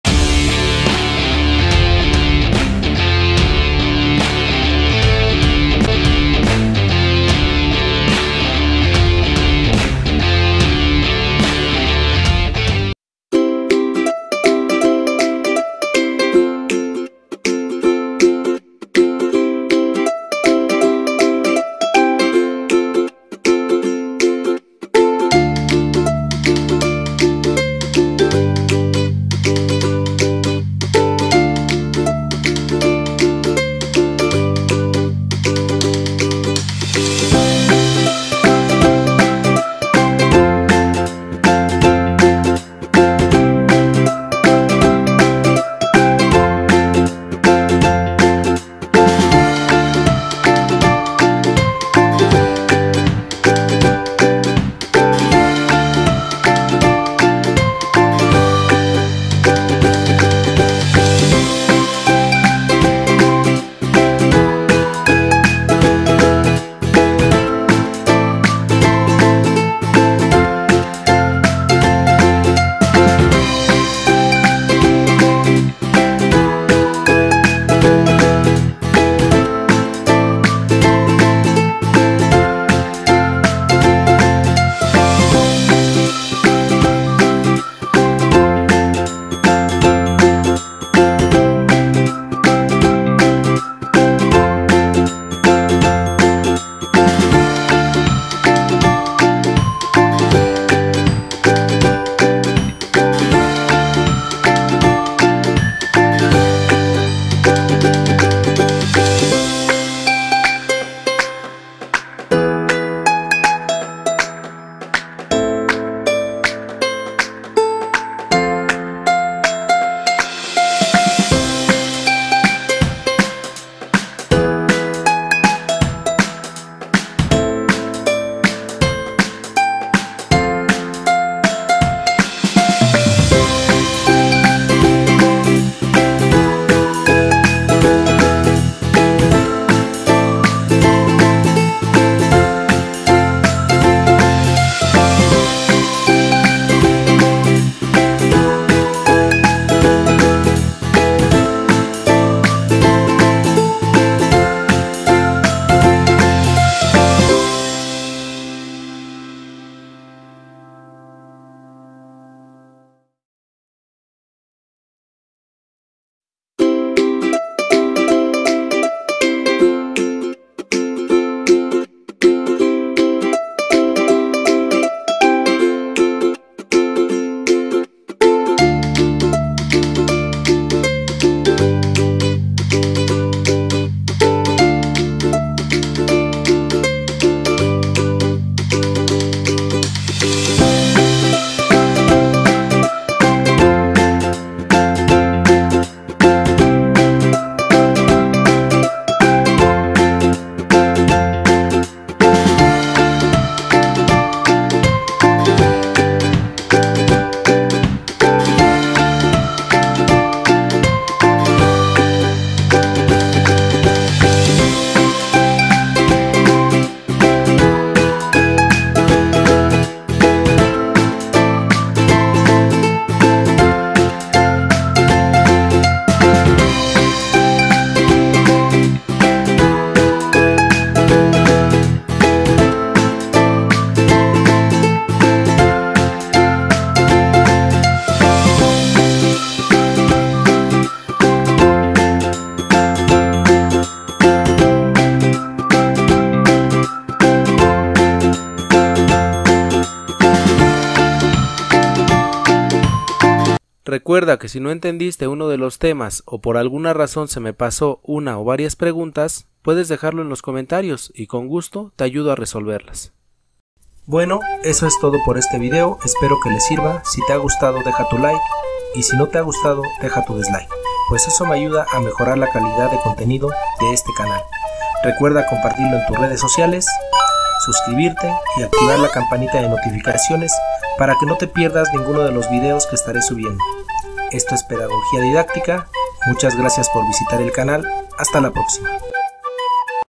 Preguntas RESUELTAS 3ro de secundaria 10 de septiembre
En este vídeo encontraras las preguntas con sus posibles respuestas del programa Aprende en Casa 2, espero que comprendan que el audio de voz quita mucho tiempo al momento de editar por eso lo pondré así de ahora en adelante, saludos.